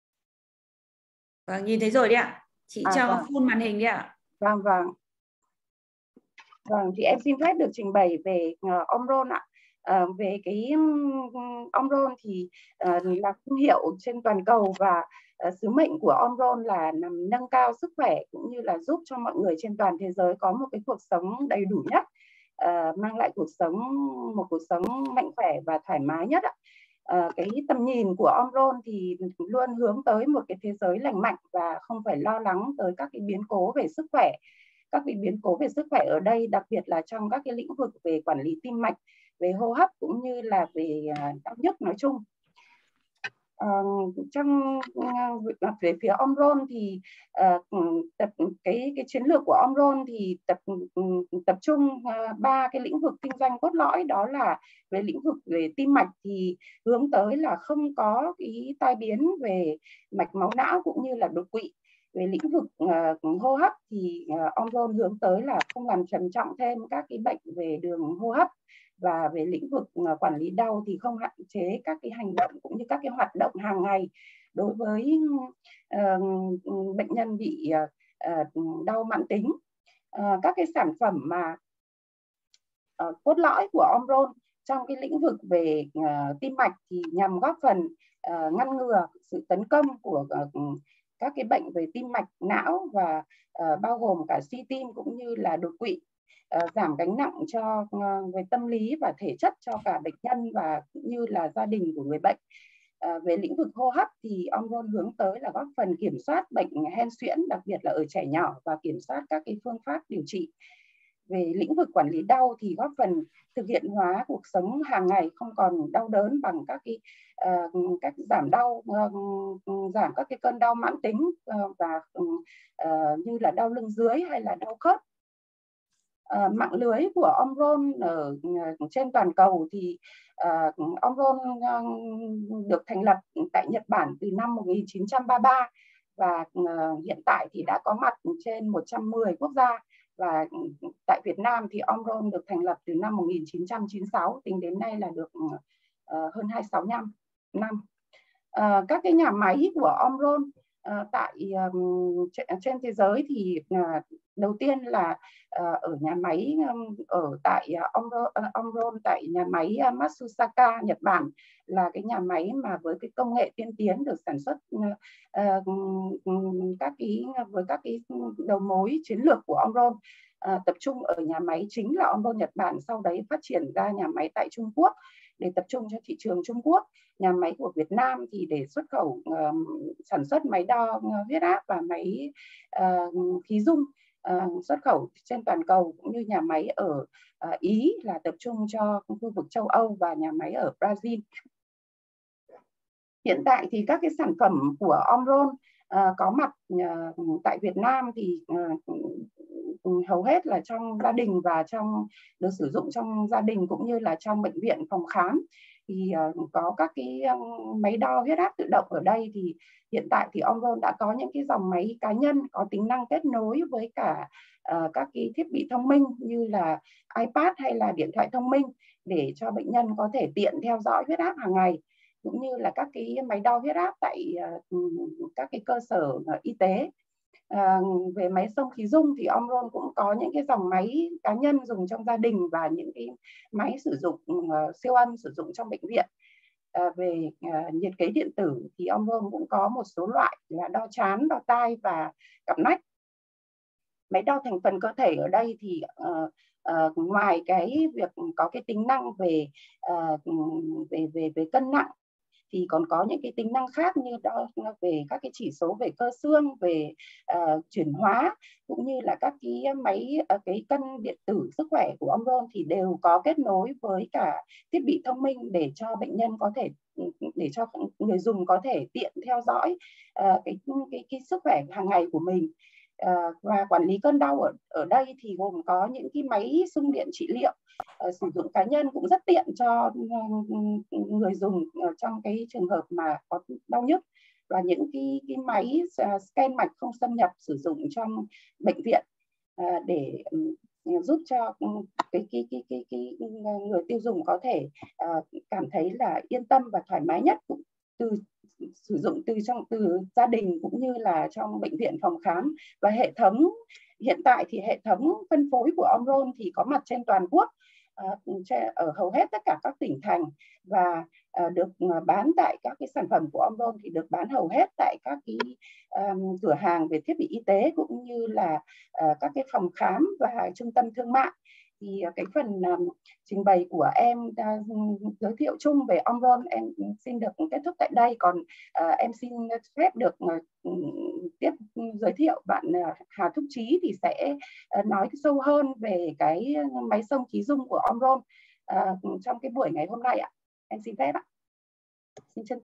GIới thiệu về OMRON
Chủ đề: Bệnh nhân bệnh phổi tắc nghẽn mạn tính và COVID-19
Báo cáo viên: ThS. Võ Thị Hồng Thái - Đại diện Công ty OMRON chi nhánh TP. Hồ Chí Minh
Đơn vị công tác: Văn phòng đại diện OMRON Healthcare Singapore pte ltd tại TP. Hồ Chí Minh